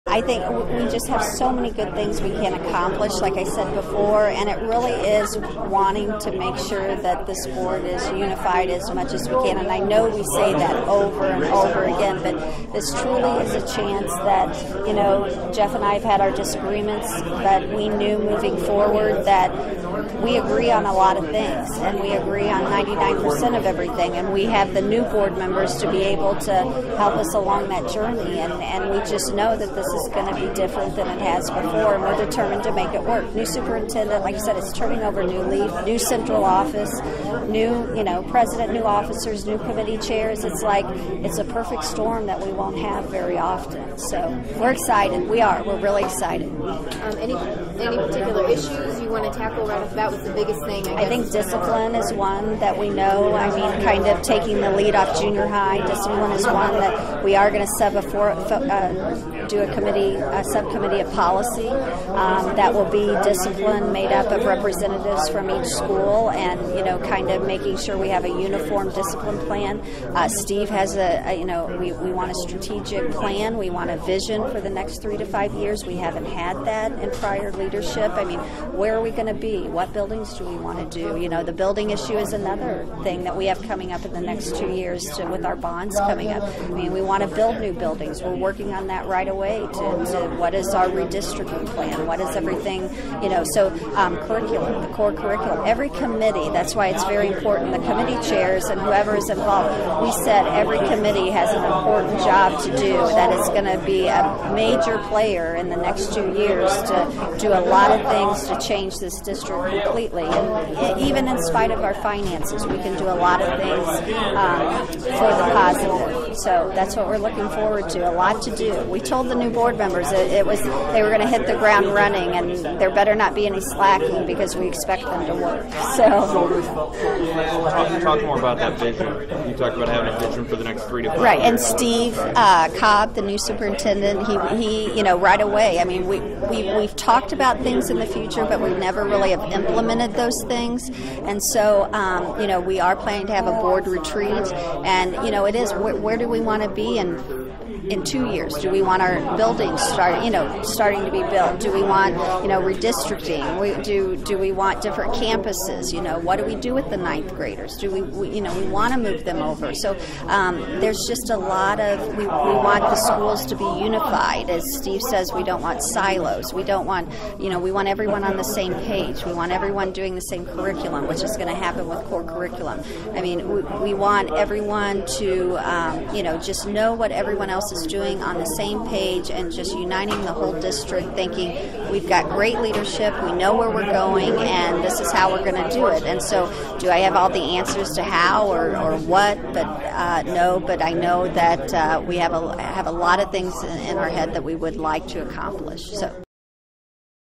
Quincy School Board President Stephanie Erwin 4-25-13
New Quincy School Board President Stephanie Erwin talks about vision for the future.